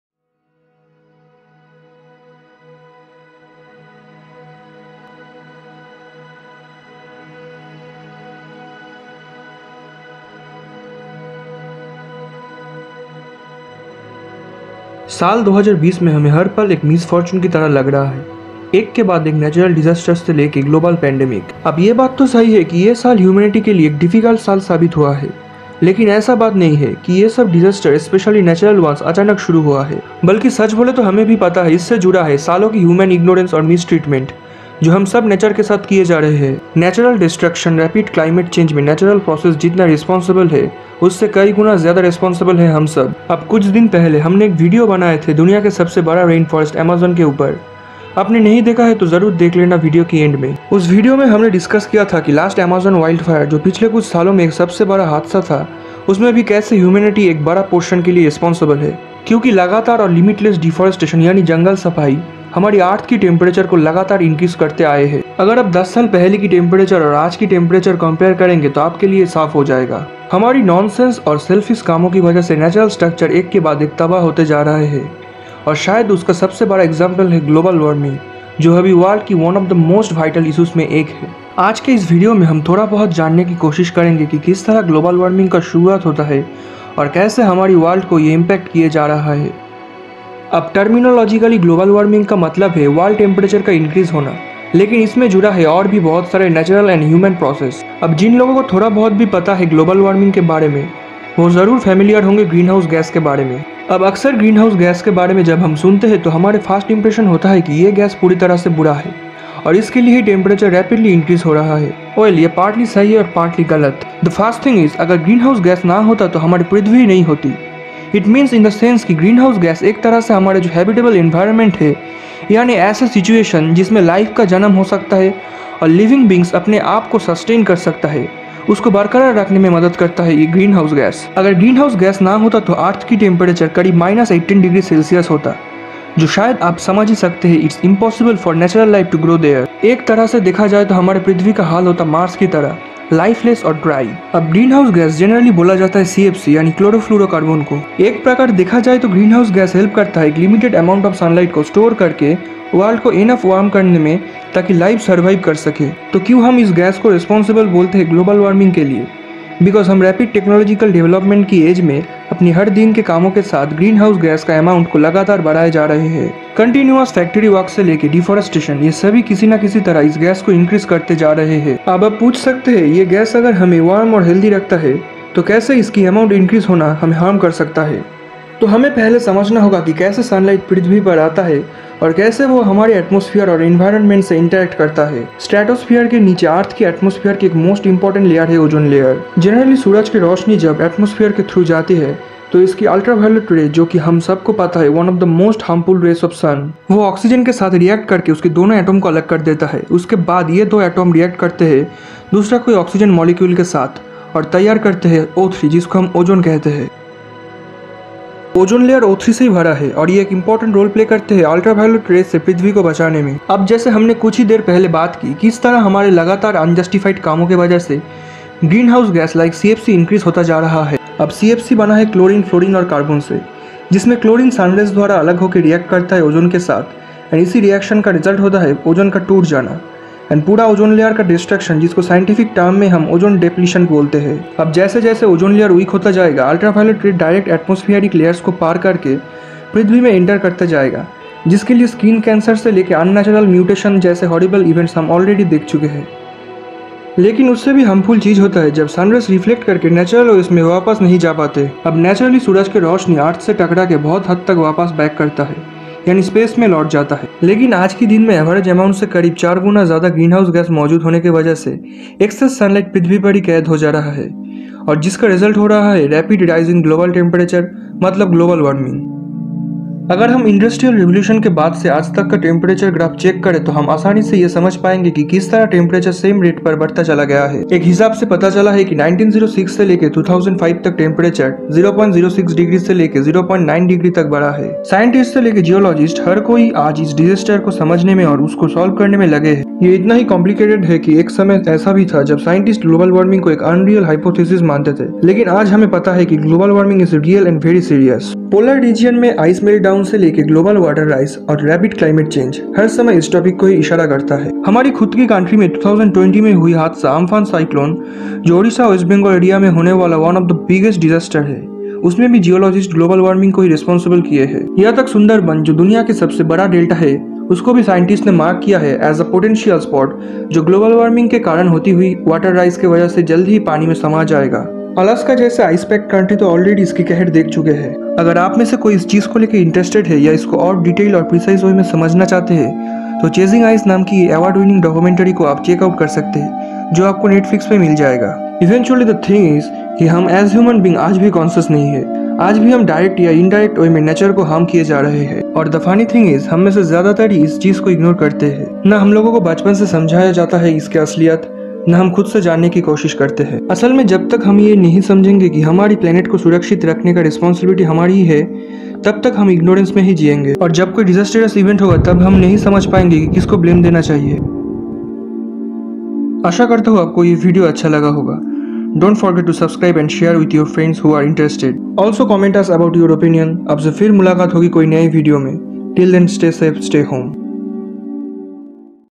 What Is Global Warming In Hindi |ग्लोबल वार्मिंग कैसे होता है | Climate Change 2020
Hey Guys, here is our new video. The topic is super vital for us and our planet. Guess you have got the topic already. Yes, it is about Global warming. Global warming in scientific terms can be horrible to understand as the scientists themselves haven’t been able to solve everything about it but don’t worry we have simplified the topic as much as we could and presented every vital part of it in a down-to-earth language. Watch the video, we hope you will understand what is the reality we are missing in our daily life, the big reality that is basically ripping our planet apart without our knowledge.

Planning : Souvik, Samrat
Voice: Rituraj
Video: Samrat

National Geographic

any copyrighted content is found tell us at 📧